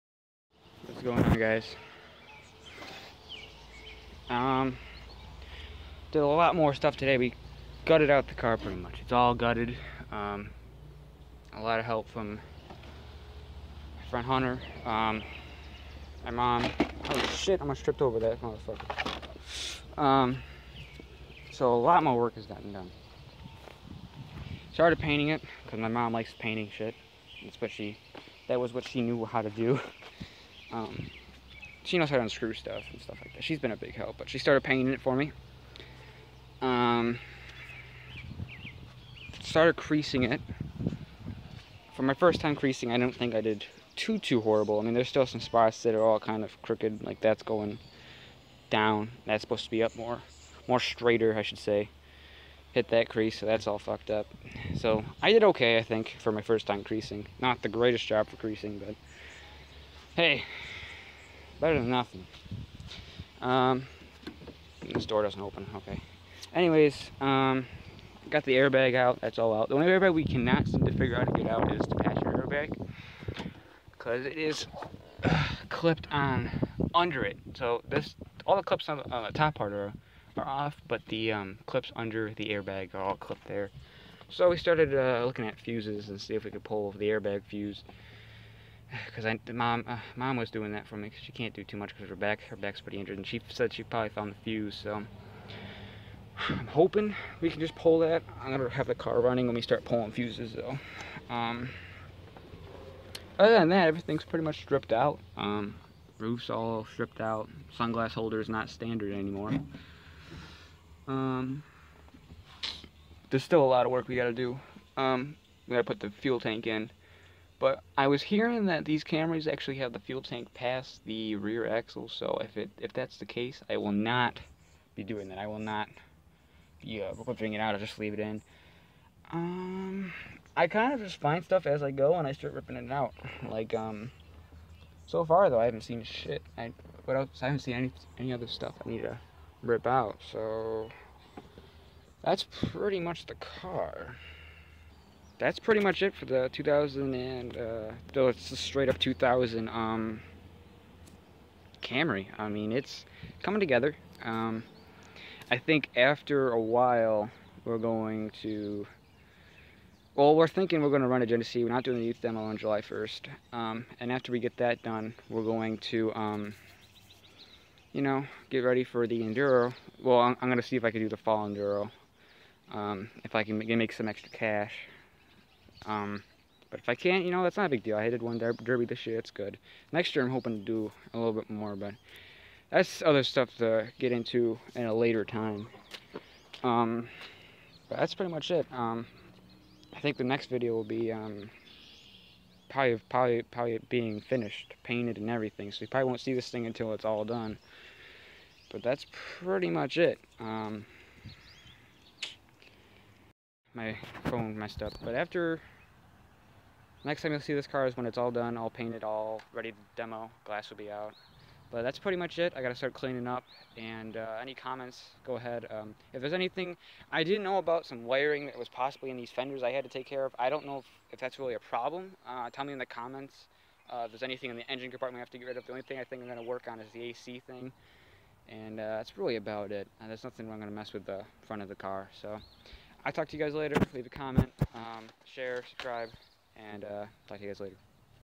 2017 Demolition derby car update 2
Next time u will see the car is win its all finish also .. my demo is july 21st 7pm at Genesee county fair wish me luck.